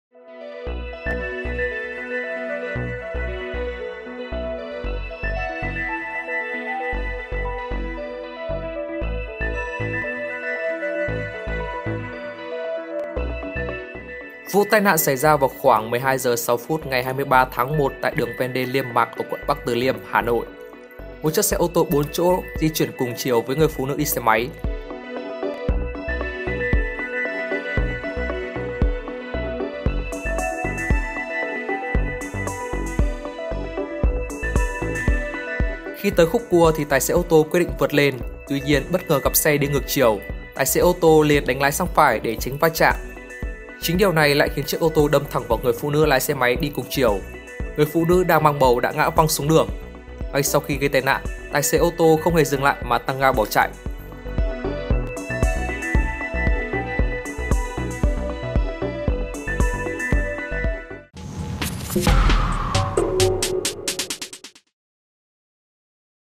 Cua ẩu húc trúng bà bầu, tài xế ô tô khiến tất cả phẫn nộ vì hành động ngay sau đó
Sau khi xảy ra va chạm , tài xế xe 4 chỗ tăng ga bỏ chạy để mặc nạn nhân